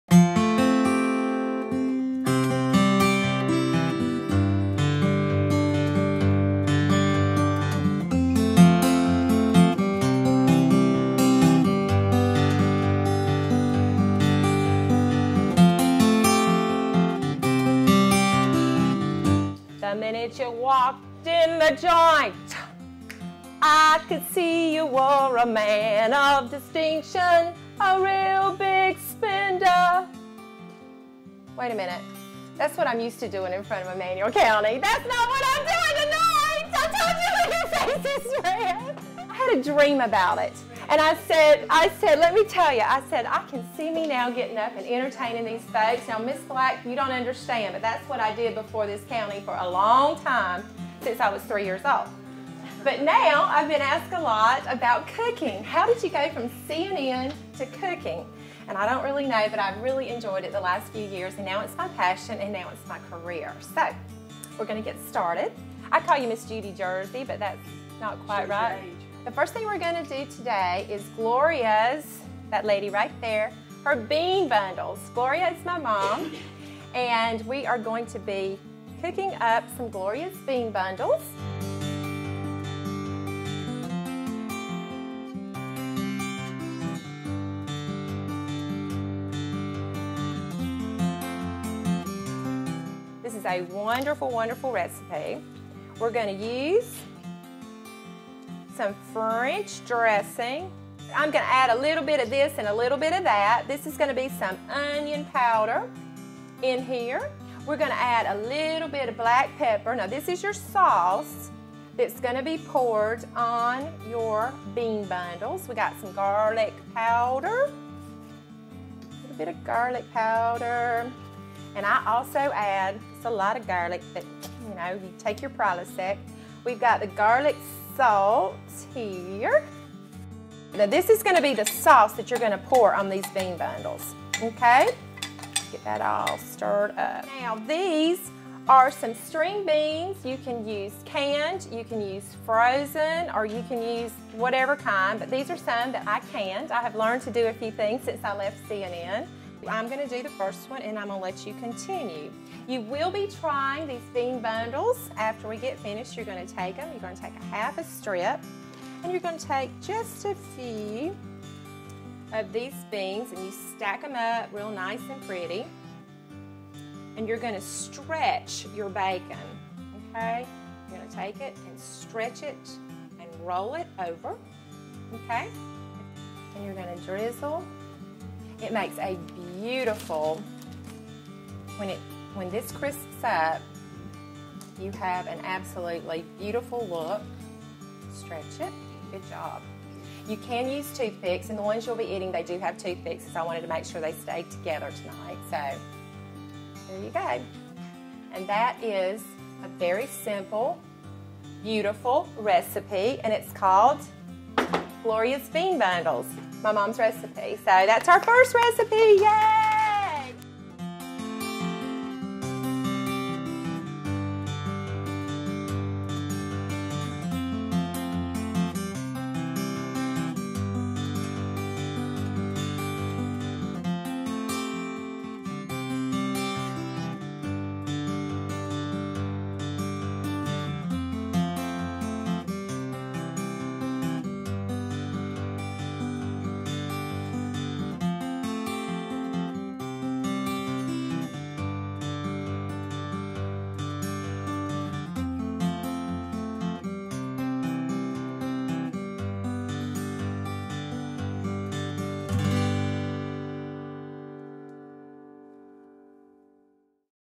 Sweet Melissa's Catering: Gloria's Bean Bundles
Melissa Kitchens visits her hometown and demonstrates how to make "Gloria's Bean Bundles."